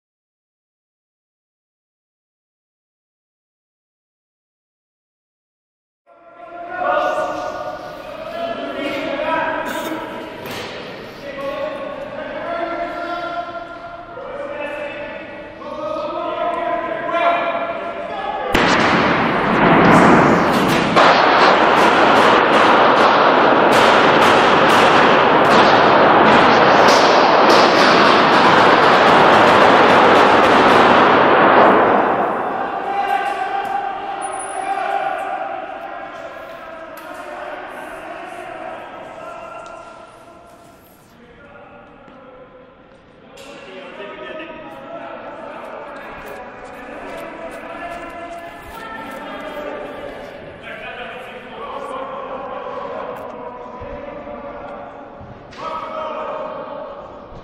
Inside Parliament in Ottawa during 2014 shooting
On October 22, 2014 a gunman opened fire at the War Memorial in Ottawa and then had a shootout with police inside Centre Block of Parliament. A Globe and Mail journalist was inside Parliament and captured this footage where an exchange of gunfire can be heard.